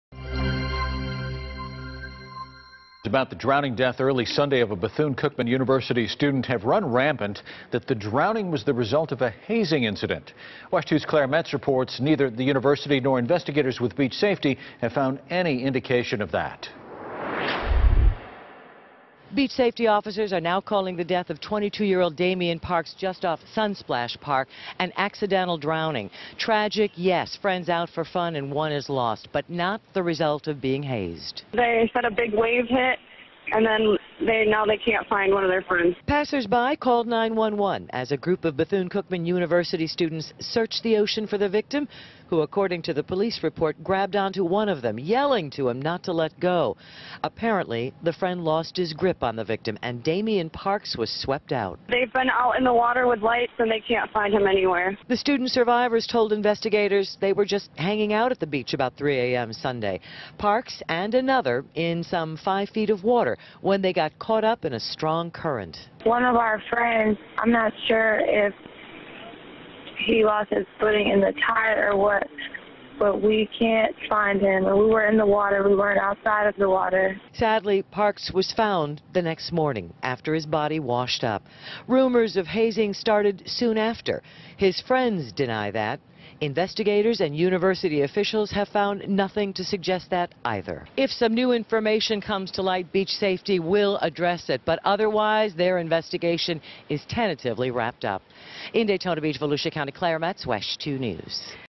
Officials say no indication of hazing in B-CU student's death
WESH 2's Claire Metz (@ClaireMetzWESH) reports, neither the University nor investigators with beach safety have found any indication of that.